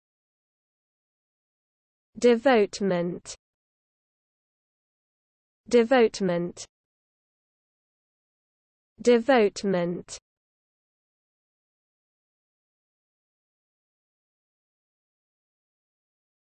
How to pronounce Devotement (CORRECTLY)
Learn how to correctly pronounce the word Devotement  in the US English accent.